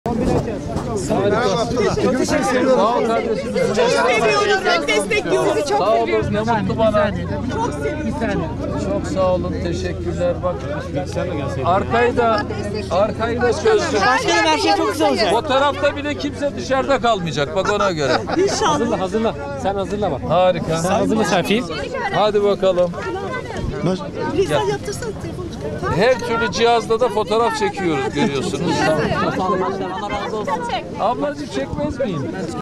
Tabletle fotoğraf çeken İmamoğlu'ndan esprili yorum "Görüyorsunuz her türlü cihazla..."
İstanbul Büyükşehir Belediye (İBB) Başkanı Ekrem İmamoğlu “İSKİ Yenikapı Havzası Atıksu, Yağmur Suyu ve Dere Islah Yatırımları Temel Atma Töreni”ne katıldı. Tören sonrası gazetecilerin sorularını yanıtlayan İmamoğlu, ardından İstanbullularla fotoğraf çektirdi.